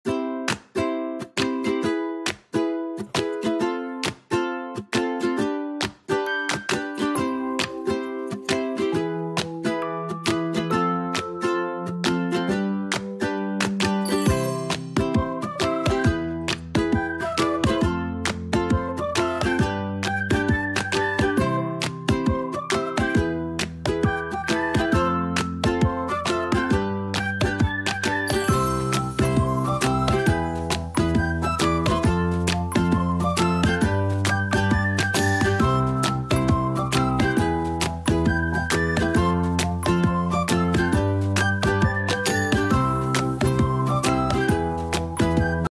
Old Photo Restoration Tutorial: Repair Damaged Photos By Using Manual-Fix In Seconds
In this video, I show you how I manually paint out the scratches using Nero AI Photo Restoration.